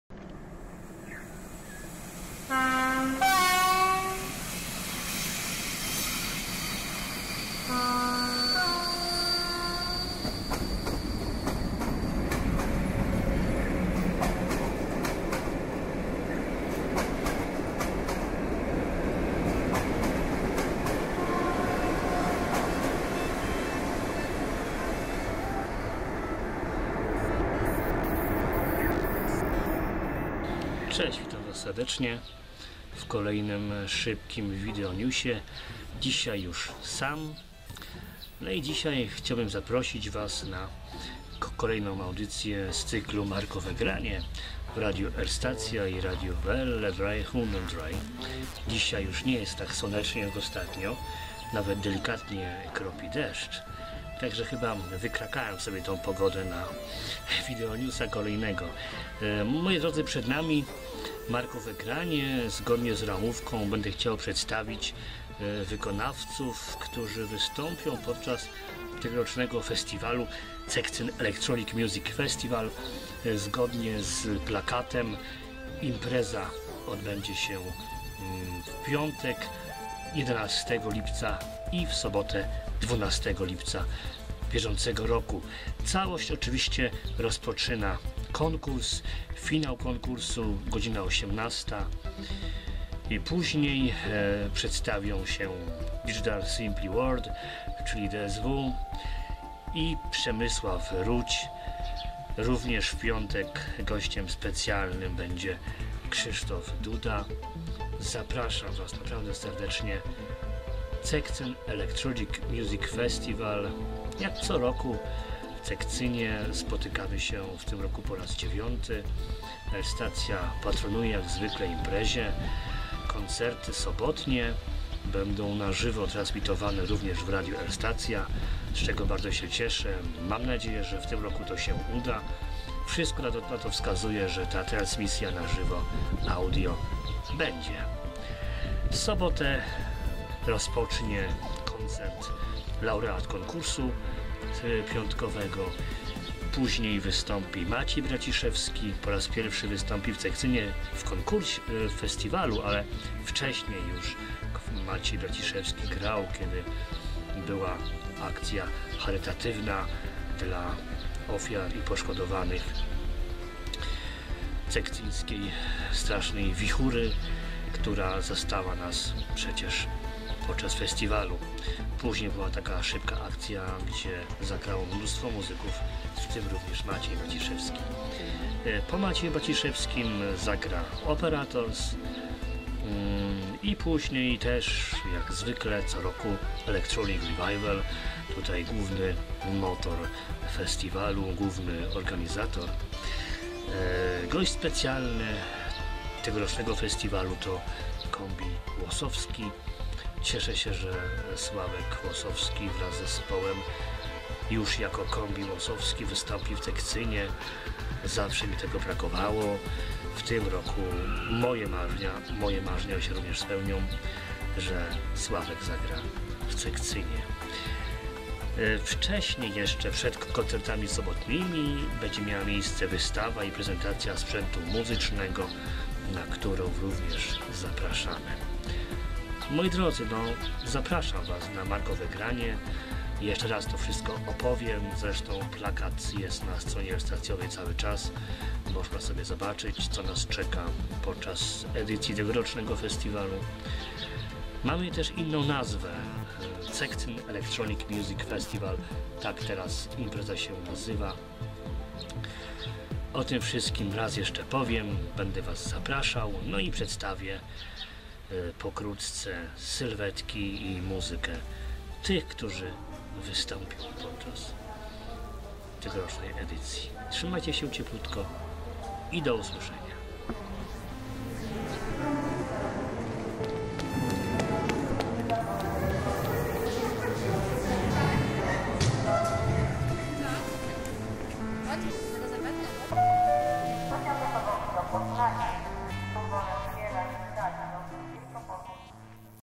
VideoNews | Plener | 06 | 2014.06.21
Kolejny videonews zapraszający Was do słuchania Markowego Grania.
Tym razem o CEMF czyli Cekcyn Elektronic Music Festival